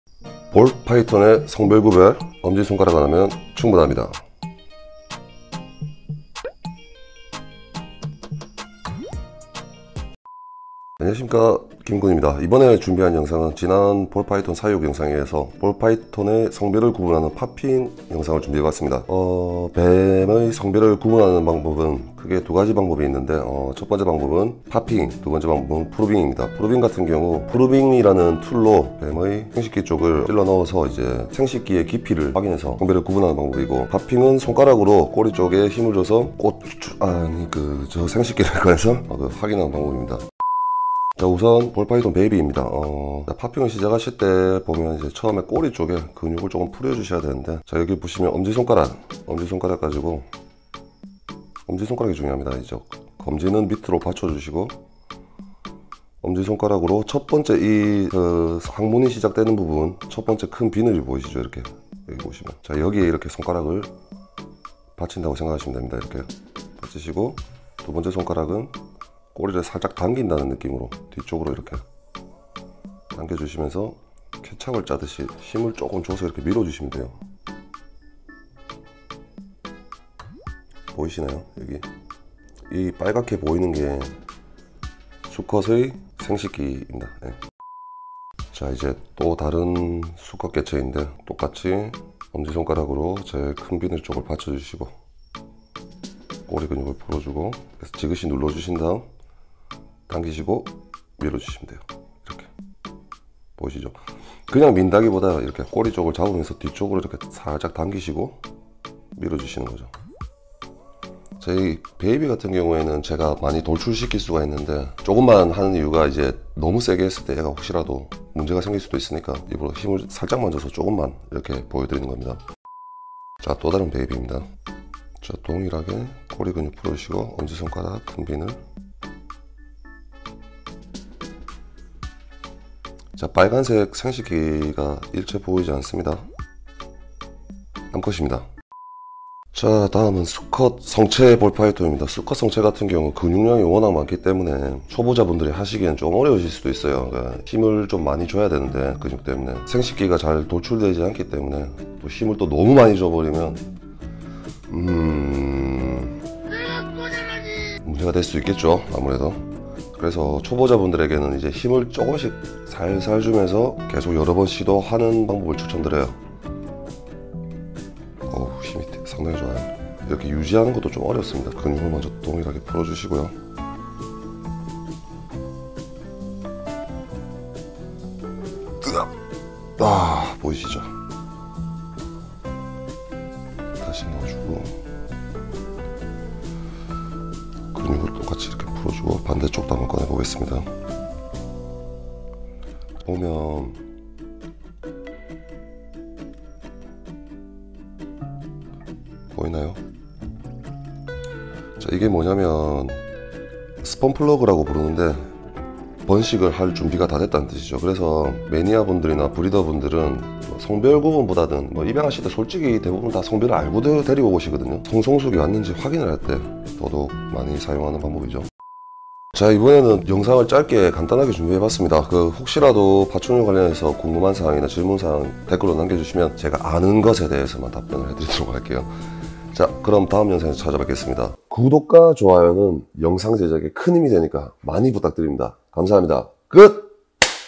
애완비단뱀 볼파이톤 팝핑. 손가락만으로 성별 구분하는 법. 볼 파이톤 #2
애완비단뱀 볼 파이톤의 성별구분 방법인 "팝핑"에 대한 영상입니다.

파충류톡 김군의 정보